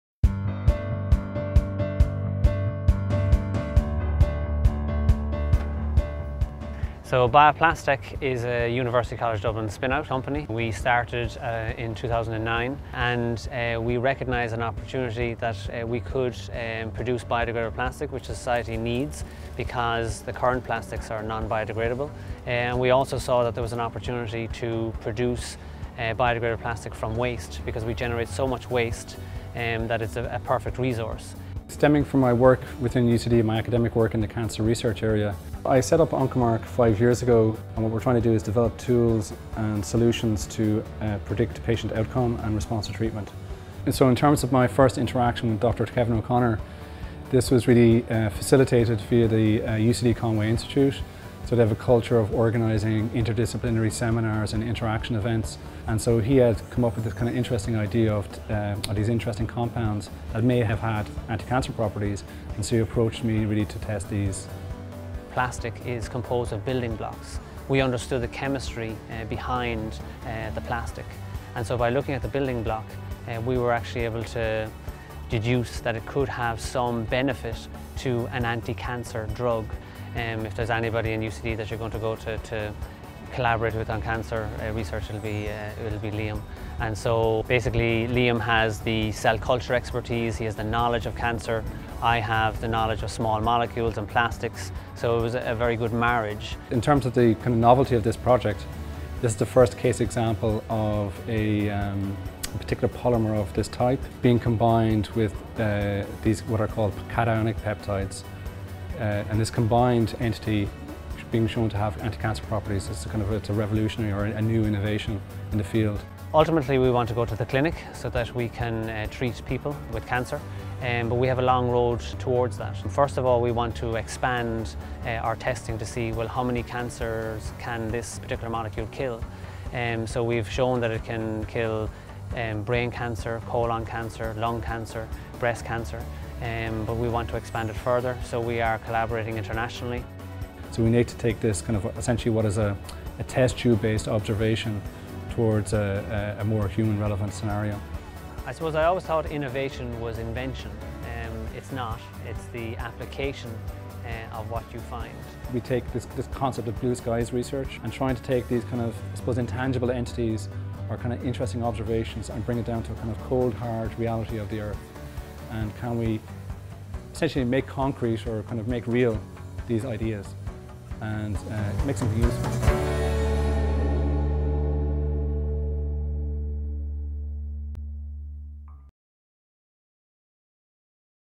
Fighting Cancer with Biodegradable Plastic Molecules
This UCD Innovation video focuses on the research by two UCD researchers, Dr Kevin O'Connor and Professor William (Liam) Gallagher, into fighting cancer with biodegradable plastic molecules.

Dr Kevin O'Connor, UCD School of Biomolecular and Biomedical Science, is the founder of Bioplastech, a UCD spin-out company that has developed a process to convert waste materials to an environmentally friendly plastic.

Professor William Gallagher, UCD School of Biomolecular and Biomedical Science, is a co-founder of OncoMark, a UCD spin-out company that develops tools and solutions to predict patient outcome and response to cancer treatment.

Bioplastech and OncoMark are both client companies of NovaUCD, the Centre for New Ventures and Entrepreneurs.

In this video, Dr Kevin O'Connor and Professor William Gallagher explain how the culture of interdisciplinary research at the UCD Conway Institute has facilitated them to combine their expertise to identify and develop potential anti-cancer drugs.